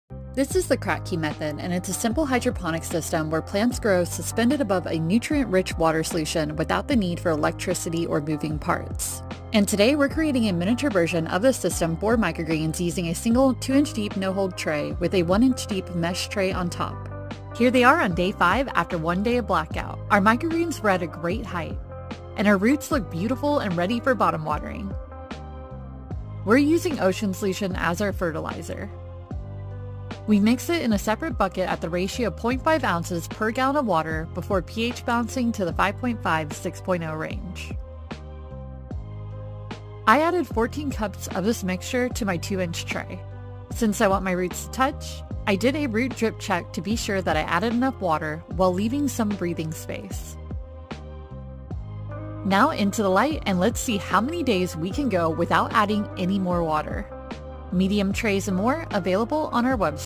P2 Mini Kratky Method Test: Grow Broccoli Microgreens with Little Electricity! 🌱💧 Silicone Medium
🌱Trays & Reusable Grow Mediums Sold Here ➽

Part 2) 🌱 💧 Testing the Kratky Method on a miniature scale for growing Broccoli Microgreens with our Silicone Reusable Grow Medium! As previously mentioned, if you aren't familiar, the Kratky growing method is a hydroponic system/method created & named after Dr. Bernard Kratky. It's a way of growing plants suspended above a reservoir that is filled with nutrient rich water. What makes Kratky different from the DWC (deep water culture) method is that there is no use of electricity (pumps & airstones). 💧🌱

✨️✨️ This simple setup can be a great way to make sure your Microgreens are watered while stepping away for a day or two without investing in an automated system. The main thing is to make sure you refresh the water every so often so that you don't run into root rot. Which, with Microgreens, doesn't occur often since the turnaround rate is quicker than, say, peppers, adult basil, or tomatoes that are more commonly grown using the Kratky Method. ✨️✨️

In part one of this series, we had seeded the medium & began germination. Now, here we are in part 2, five days into simply misting the seeds with water, and it was time to add our nutrient water to the reservoir tray. We added 14-cups of our mixture, and now we'll see how many days we can go without adding any more water at all. 🌱🌱

✨️✨️ The rest of this series is available on our Tiktok & Instagram @onthegrowfarms